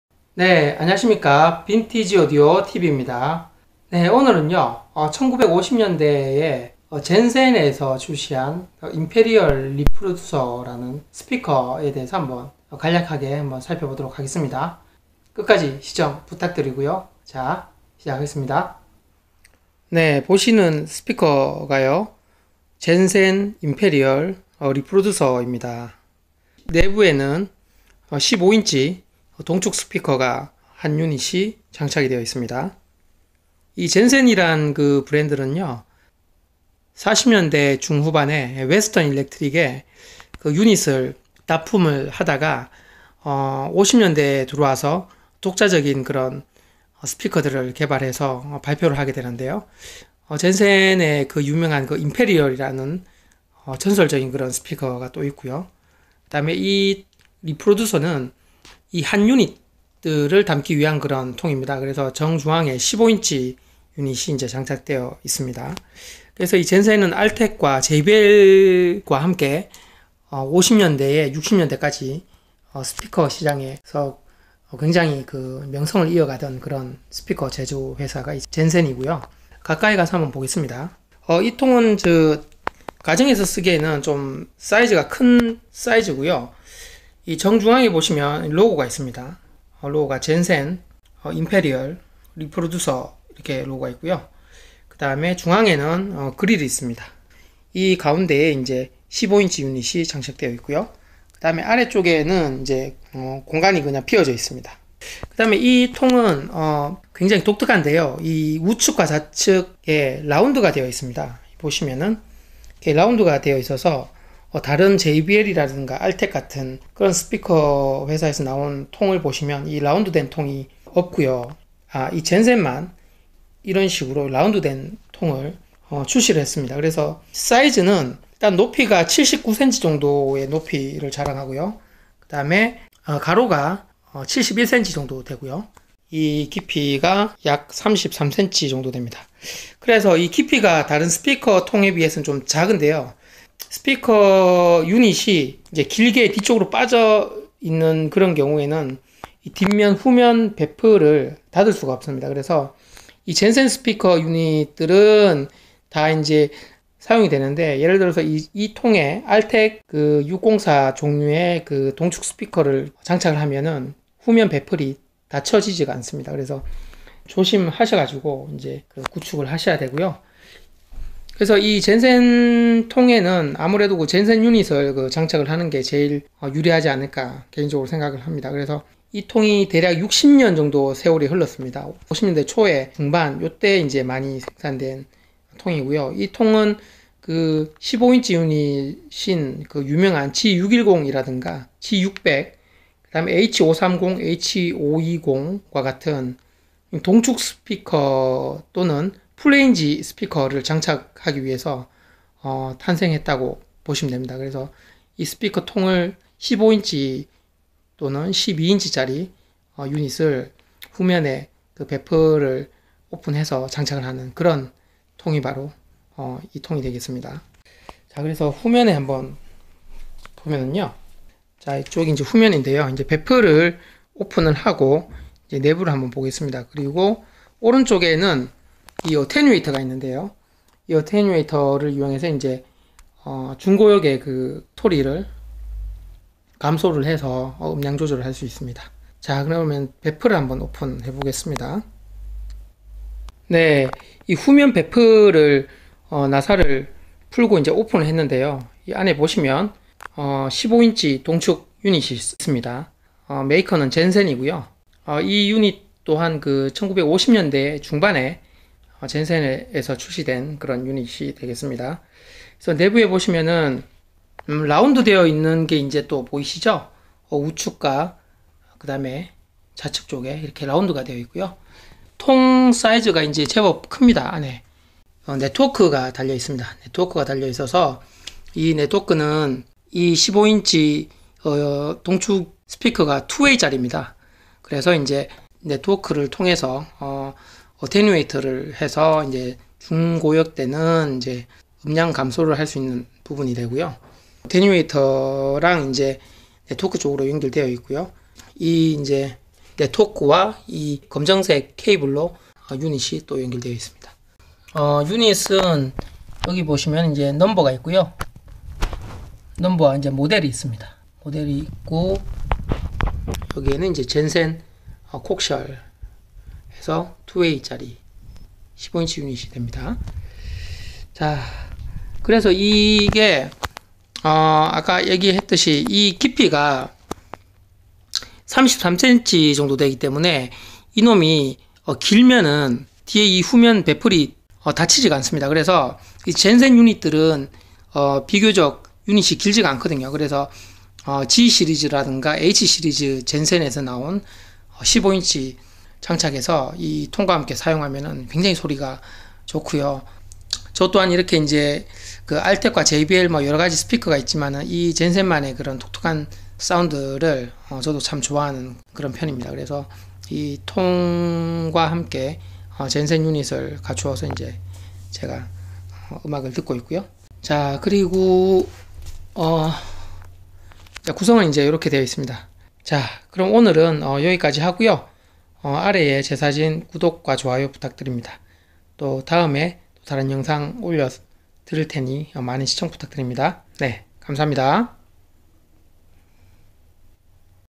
젠센 임페리얼 리프로듀서 스피커 리뷰~Jensen Imperial Reproducer Review (15 inch H530 Coaixial)~ENG subtitles
## Vintage Audio TV ##
[Vintage audio reviews, collection of audio materials, listening to healing music]
@@ Subtitles are supported in English.
Jensen imperial reproducer with 15-inch Coaxial Unit.
(1950년대 젠센 H530 15인치 동축 스피커 리뷰)